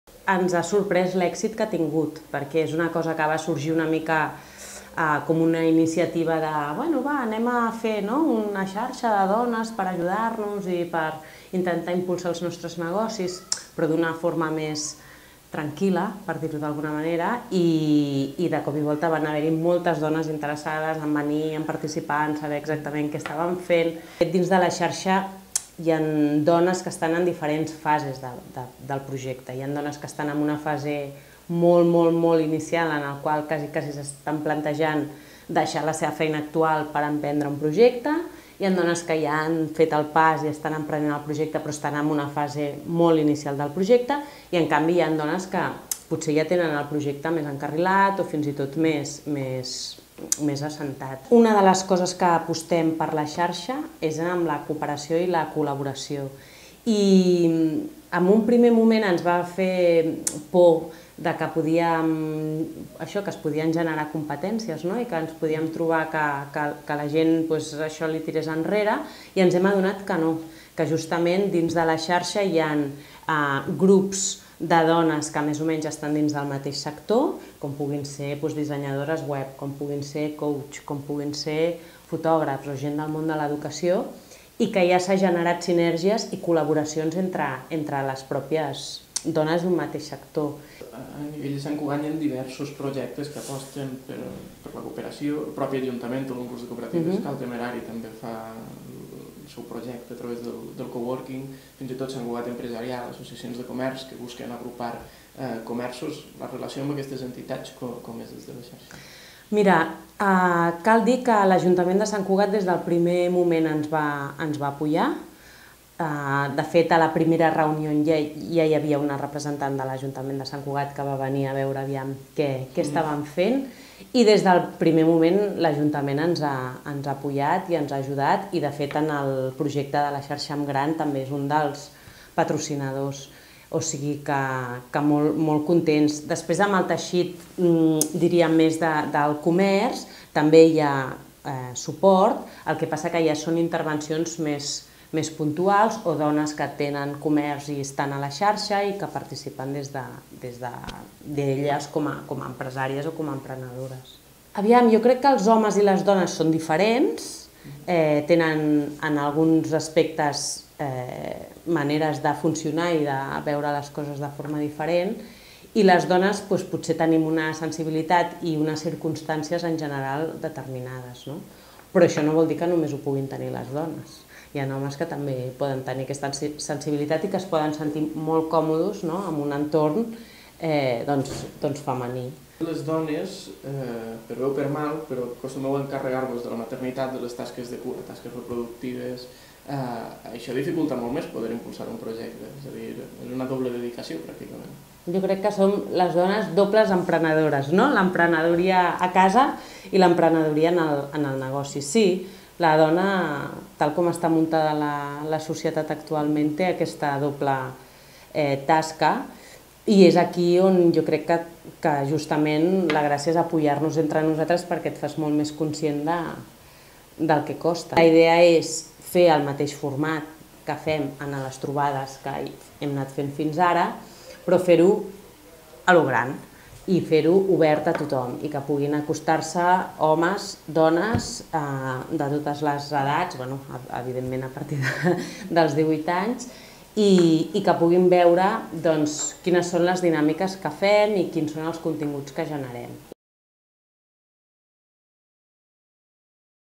Entrevista a Marta Fàbregas, presidenta de la Xarxa de Dones Emprenedores
Fa més d’un any que es va crear la Xarxa de Dones Emprenedores per fomentar la unió entre projectes incipients i que volen expandir-se. Des de llavors, les integrants del col·lectiu han fet diverses trobades en què reflexionen amb experts sobre el món empresarial i la mirada femenina dins del mercat. Ara, però, volen fer-se encara més visibles i, després de constituir el col·lectiu com a associació, han decidit fer una jornada oberta –la Xarxa en gran– per a no sòcies com un espai de cooperació entre projectes i persones interessades en la mirada femenina de l’emprenedoria.